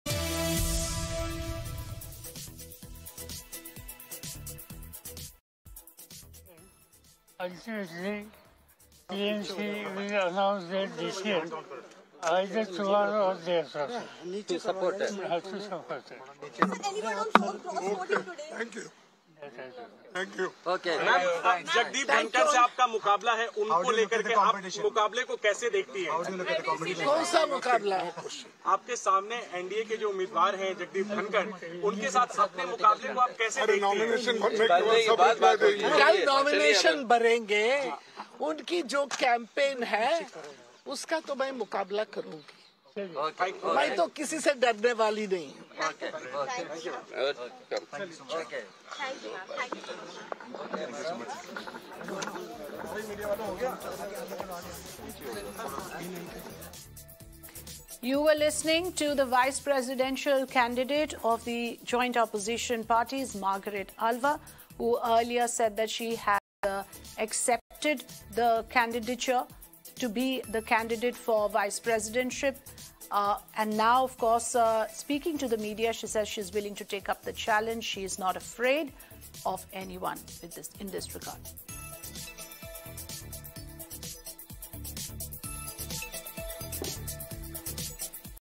Opposition Holds Strategic Meet At Sharad Pawar's Home, VP Candidate Margaret Alva Joins
On Monday, opposition leaders gathered at the home of senior politician and NCP leader Sharad Pawar. Following the conclusion of voting in the presidential election, the meeting was held. This is the second occasion in as many days that opposition party leaders have met in this manner. They convened on Sunday to decide who would serve as vice president, and Margaret Alva, the speaker of the House of Representatives, was named to fill the position. Former Bengal governor and NDA vice presidential candidate Jagdeep Dhankhar officially announced his candidacy earlier in the day in the presence of Prime Minister Narendra Modi. On Saturday, BJP leader Jagat Prakash Nadda announced his name as the NDA's candidate for the position.